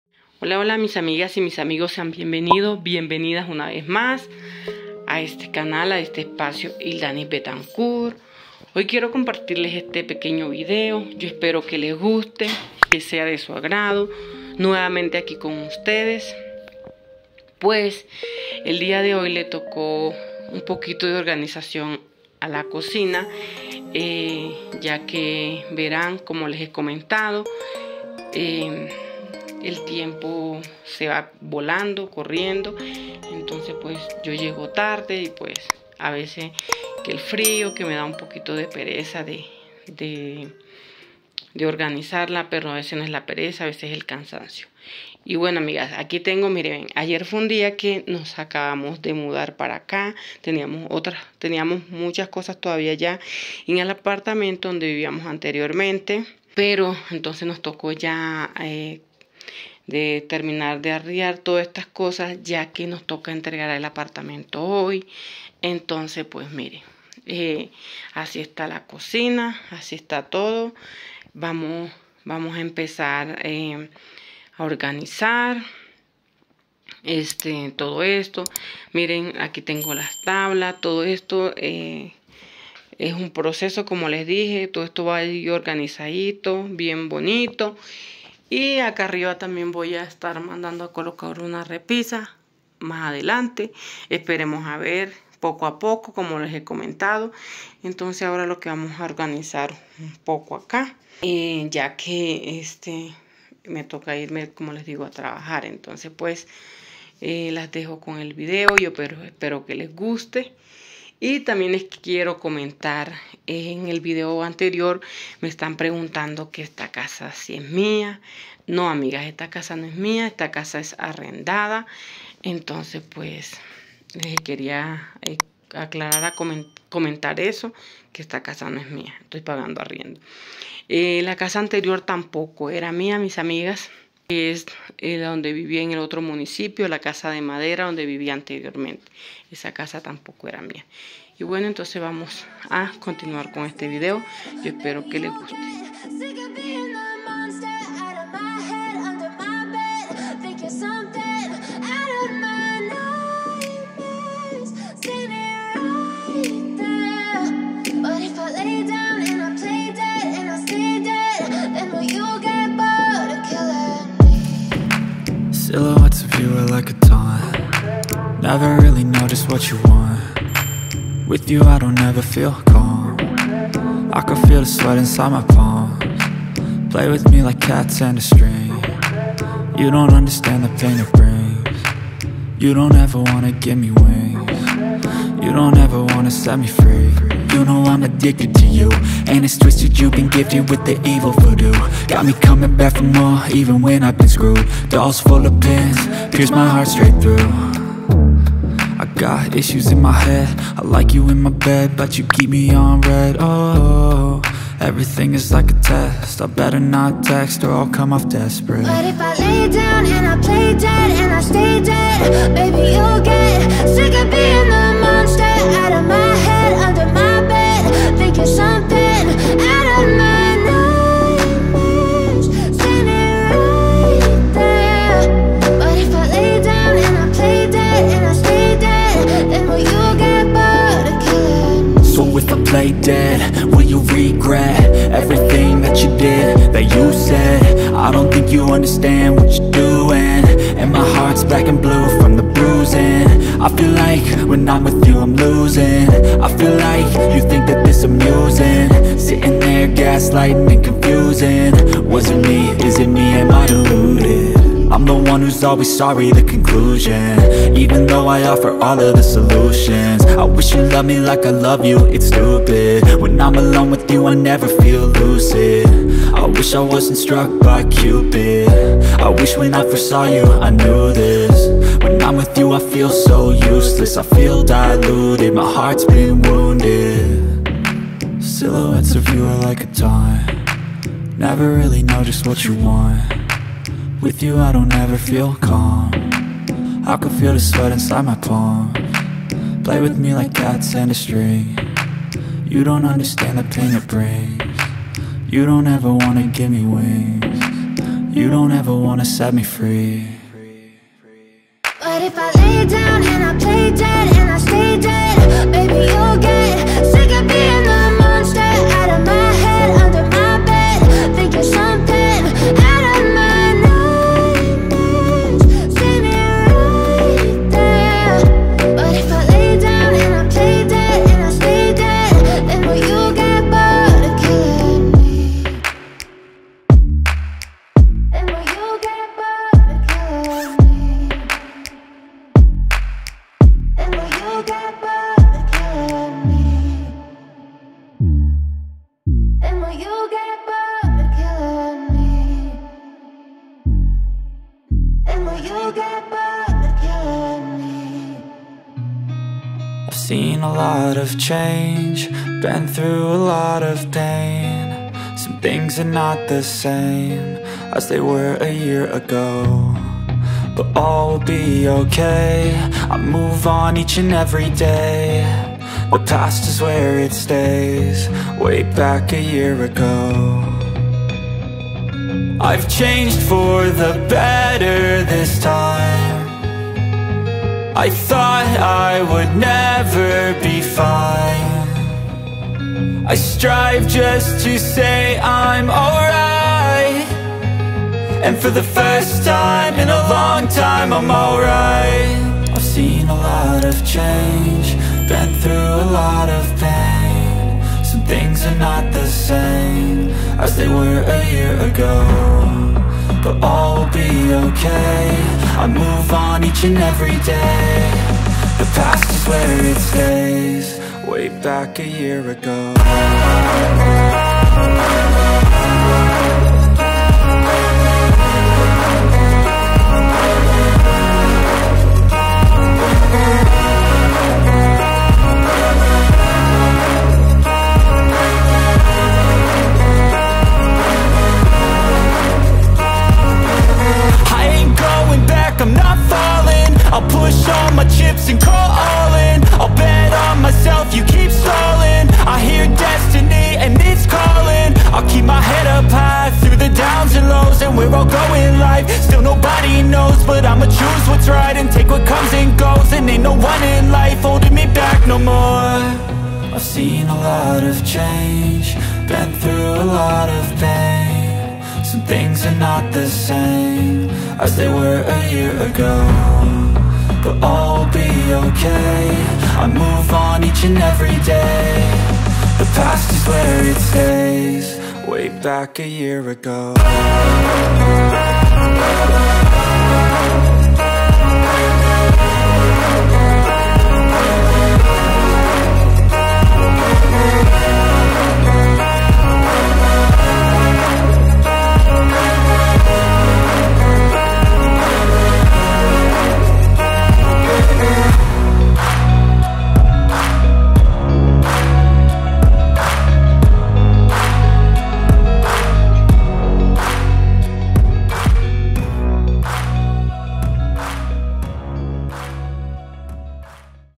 limpieza sencilla de mi nueva cocina 💯✨/ así ✅vamos organizando poco a poco 😍
Así vamos poco a poco organizando amigas ✅🫶🏽

La música de este vídeo ⬇️

Músico: @iksonmusic